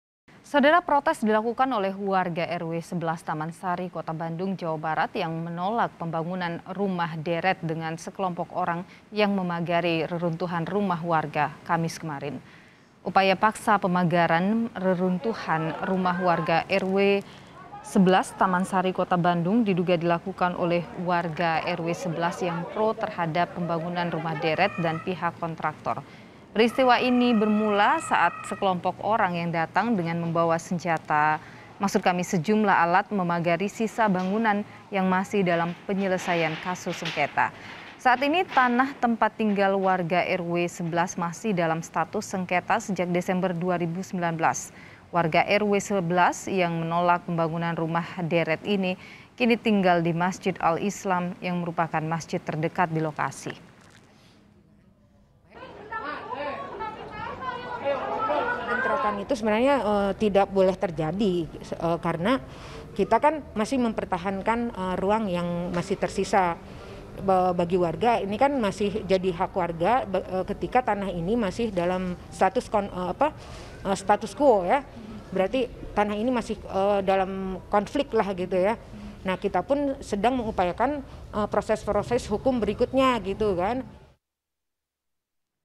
Warga Pro Pembangunan Rumah Deret dan Kontraktor Diduga Terlibat Dalam Pemagaran Paksa
BANDUNG, KOMPAS.TV - Protes dilakukan oleh warga RW 11 yang menolak pembangunan rumah deret, dengan sekelompok orang yang memagari reruntuhan rumah warga, Kamis (11/02) kemarin.

Upaya paksa pemagaran reruntuhan rumah warga RW 11 Taman Sari, Kota Bandung, diduga dilakukan oleh warga RW 11 yang pro terhadap pembangunan rumah deret, dan pihak kontraktor.

Peristiwa ini bermula saat sekelompok orang yang datang dengan membawa sejumlah alat memagari sisa bangunan yang masih dalam penyelesaian kasus sengketa.

Saat ini, tanah tempat tinggal warga RW 11 masih dalam status sengketa sejak Desember 2019.

Warga RW 11 yang menolak pembangunan rumah deret, kini tinggal di Masjid Al-Islam, yang merupakan masjid terdekat di lokasi.